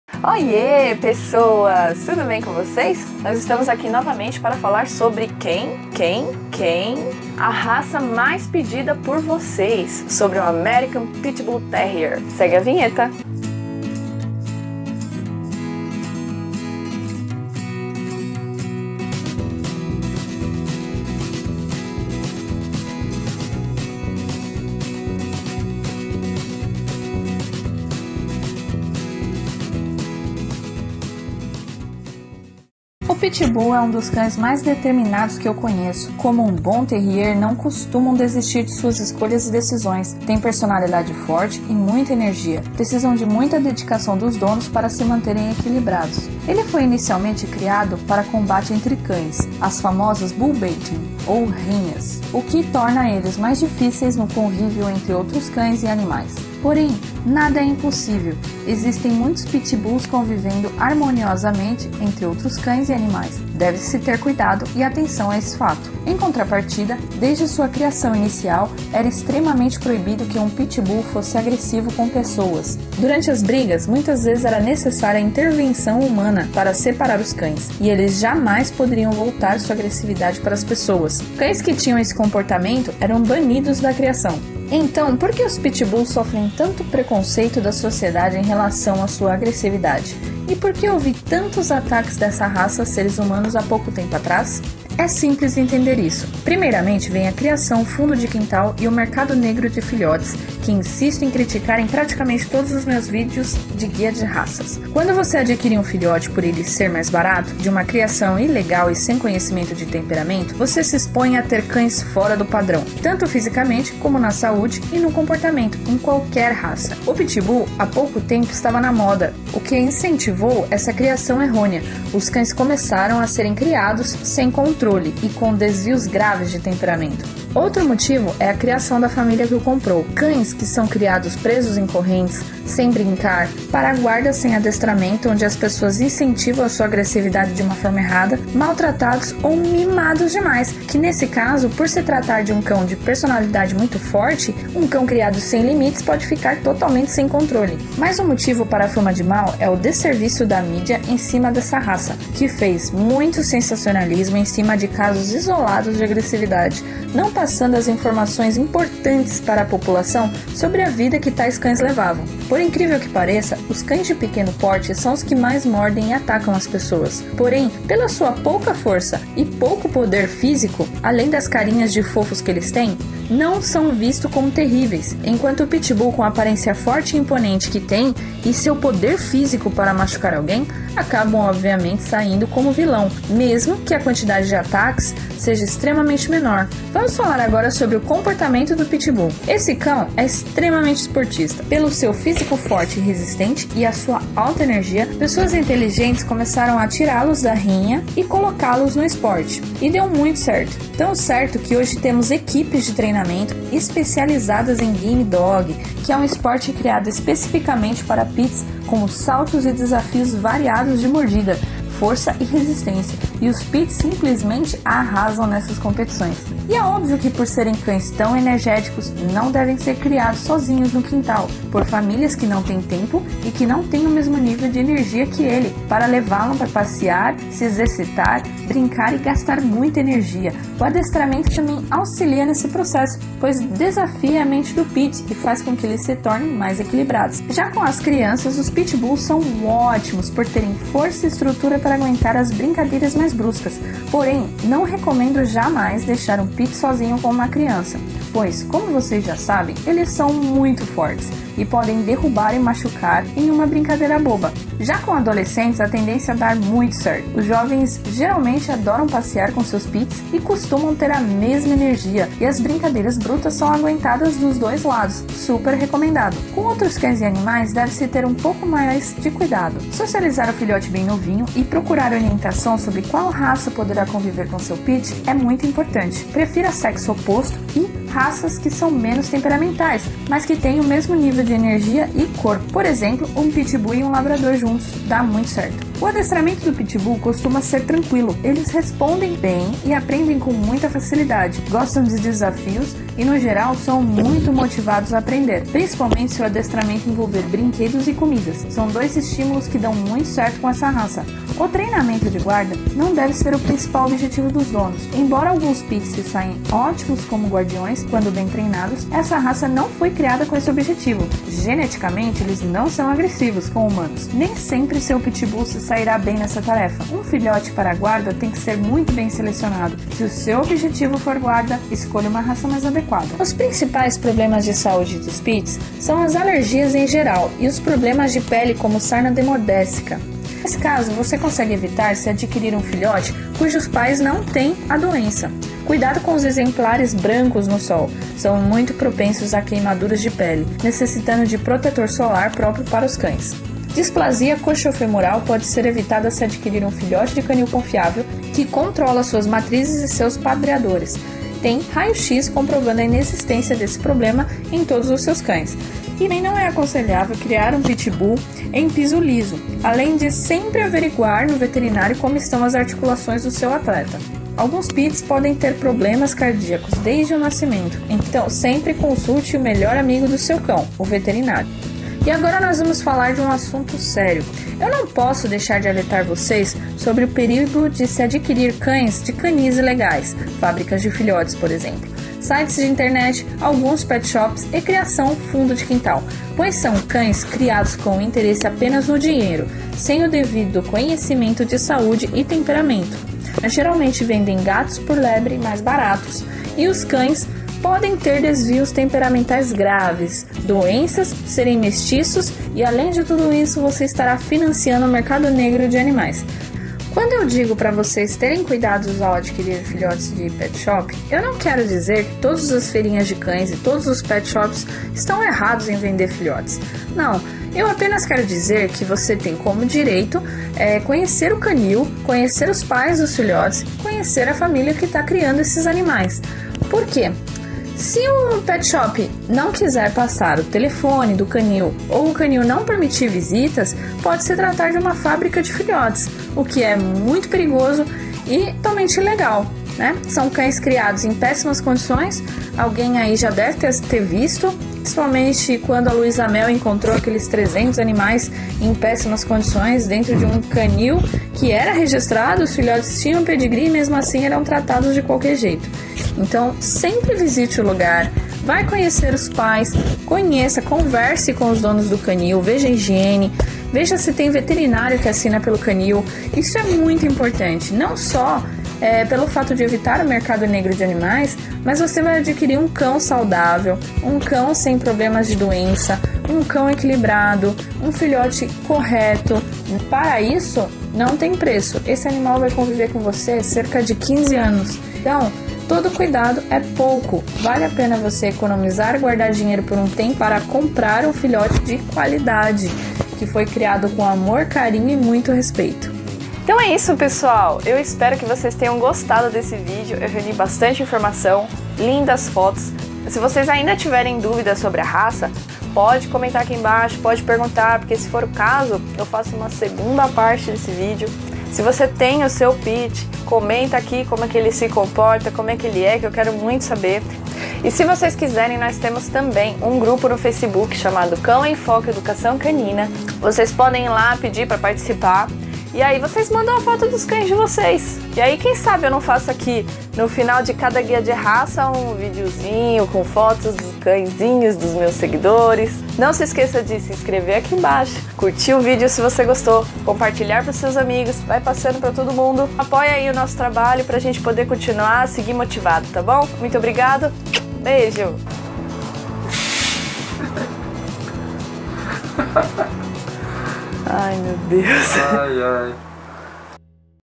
AMERICAN PITBULL TERRIER - Tudo sobre a raça
Conheça mais sobre a raça Pitbull, eles são agressivos? Porque aconteceu tantos ataques? Como eles são? Será que você pode ter? E sobre as rinhas? Como evitar fabrica de filhotes?  Saiba as respostas para essas duvidas e outras mais vendo esse video :)